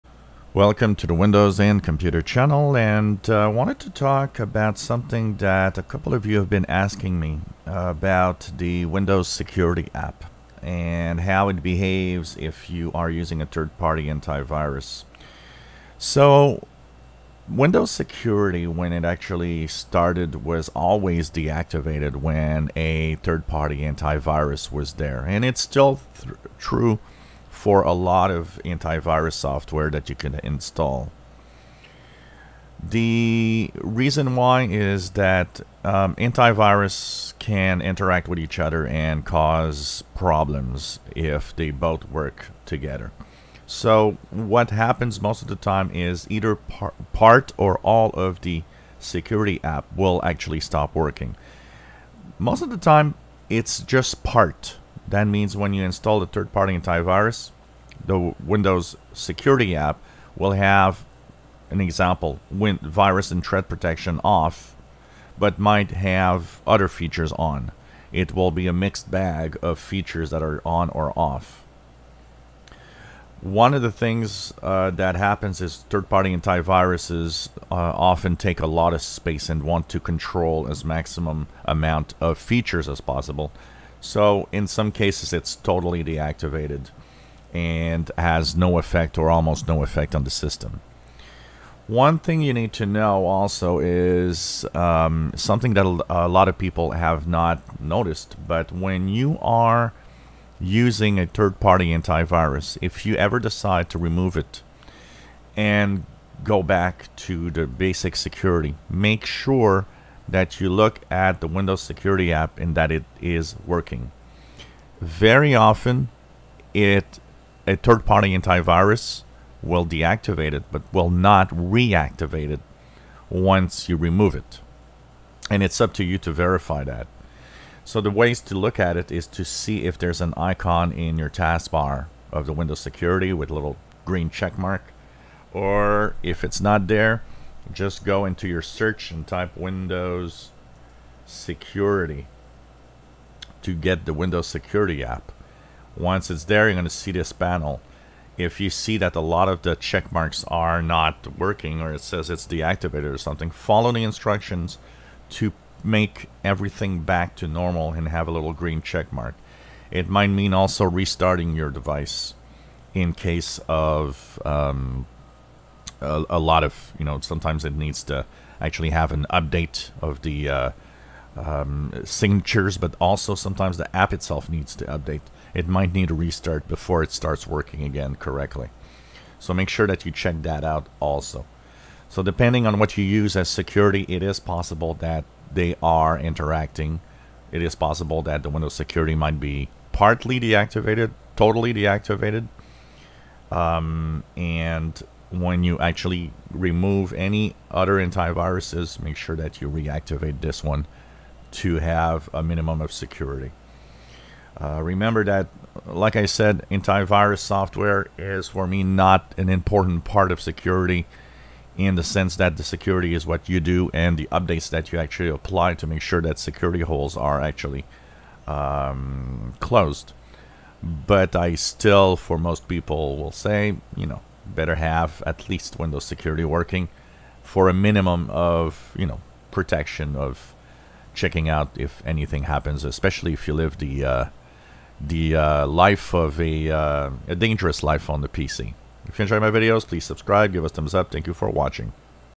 Windows Security Interactions with Third Party Antivirus software August 24th 2020
Yes Third party antiviruses might disable the Windows Security app